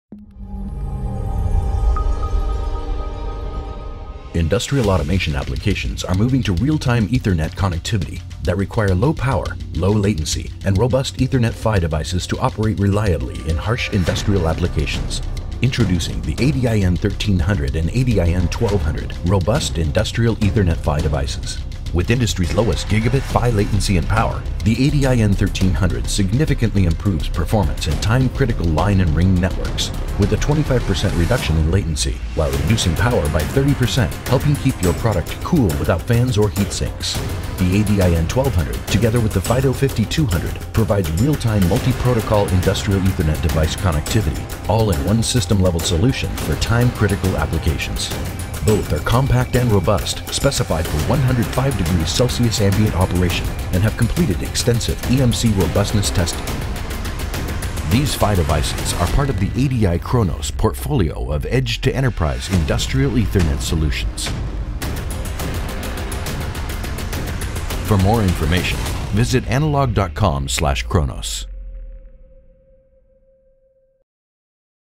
ADI Chronous ADIN1300/1200 Robust Industrial Ethernet PHYs by Analog Devices
This video introduces Analog Devices ADIChronous ADIN1300 and ADIN1200 Ethernet transceivers for Industrial Ethernet applications. They provide low power, low latency, and robustness to operate reliably in harsh environments.